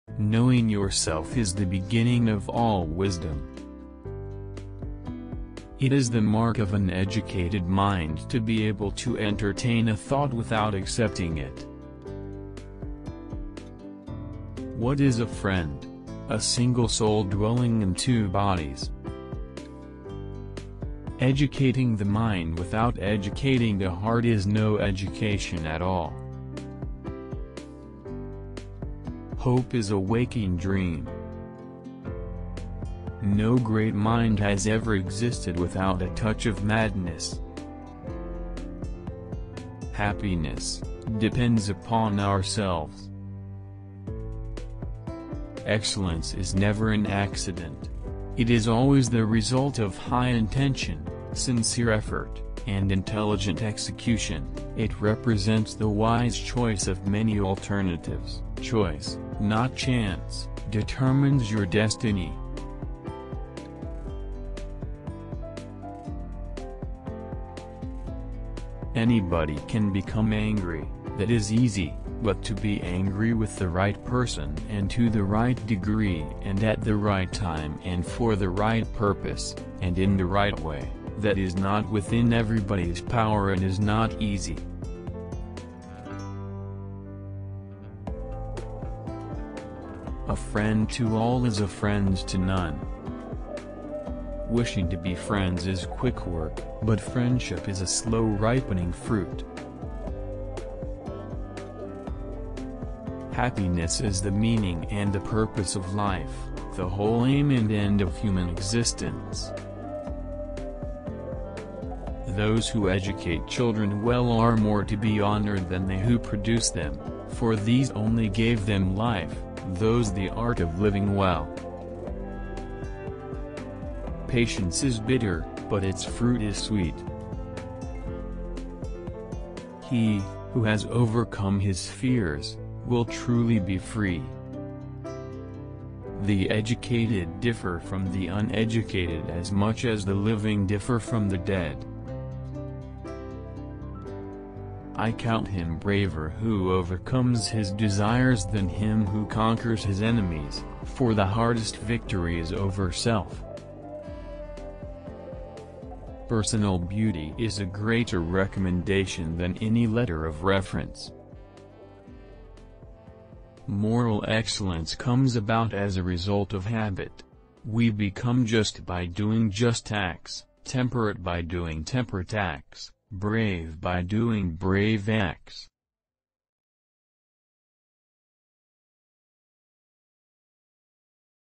Top trending quotes from the philosopher Aristotle
Aristotle (384-322 B.C.) is a Greek philosopher and polymath of antiquity. Along with Plato, whose disciple he was at the Academy, he is one of the most influential thinkers the Western world has known. He is also one of the few to have tackled almost all the fields of knowledge of his time: biology, physics, metaphysics, logic, poetics, politics, rhetoric, ethics and, occasionally, economics. For Aristotle, philosophy, originally "love of wisdom", is understood in a broader sense as the search for knowledge for its own sake, questioning the world and the science of sciences.